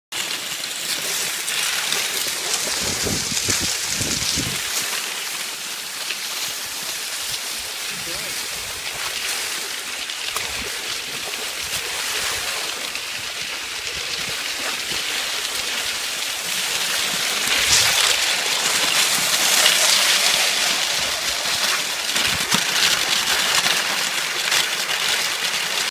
Malinois putting the power down
Jet showing that she doesn't like being in 2nd place. Watch how she puts the power down to get back up front.